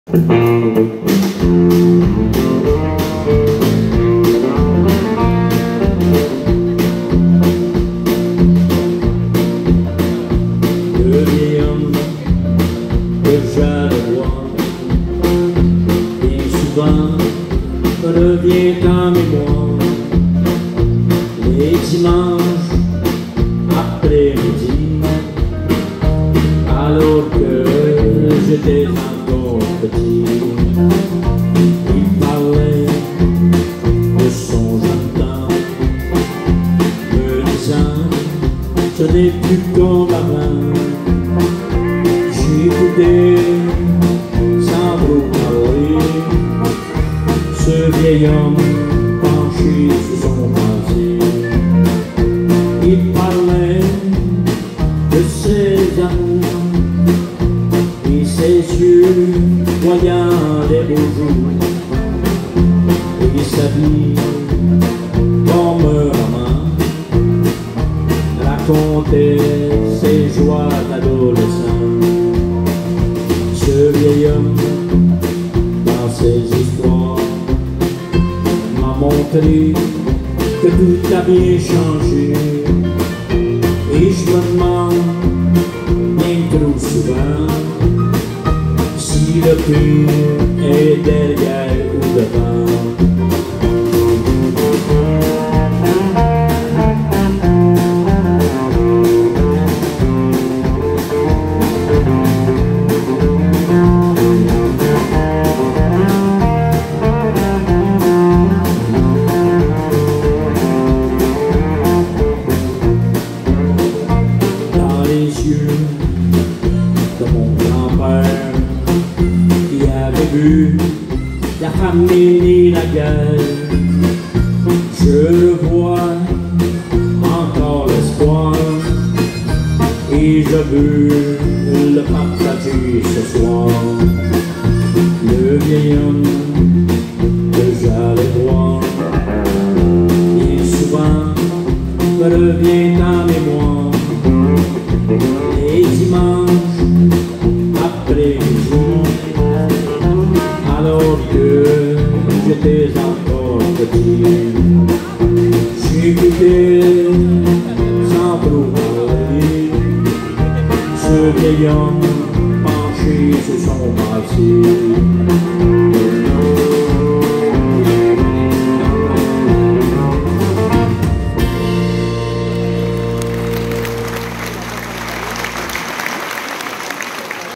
Bernard Paquette, lors du Festival de Steeve Desmarais 2018 à Notre-Dame de Pierreville
Vidéos fait par le Ti-Bout, lors du Festival de Steeve Desmarais 2018 à Notre-Dame de Pierreville ... Ne pas oublier de vous abonnez à ma chaine YouTube gratuite, en cliquant sur ma petite photo en bas à droite, merci d'avance ...